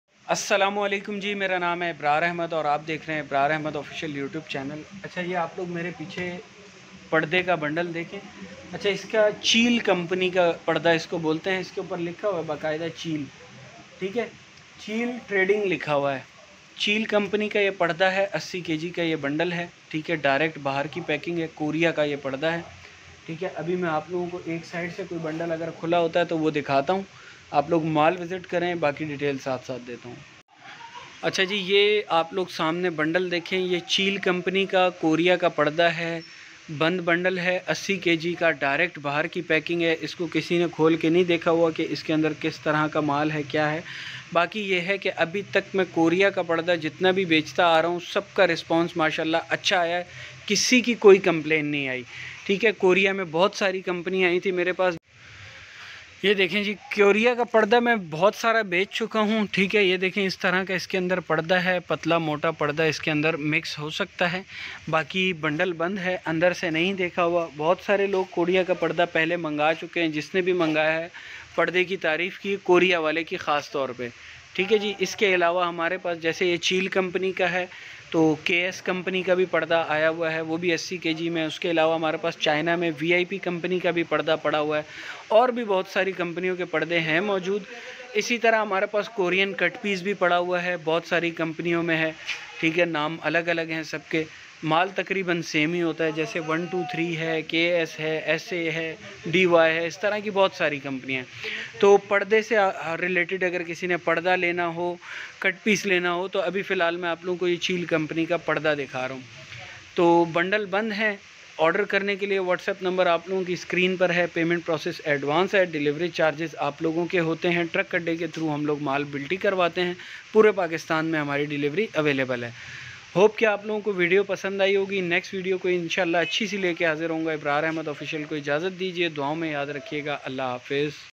Shershah Korean Curtains Bundle Cheel Company | Imported Fancy Curtains | Ibrar Ahmed Official
Hi my name is IBRAR AHMED I'm from Karachi subscirbe my channel for more videos
Toady i Will Show You Shershah Korean Curtains Bundle | Imported Curtains Fancy Curtains | Ibrar Ahmed Official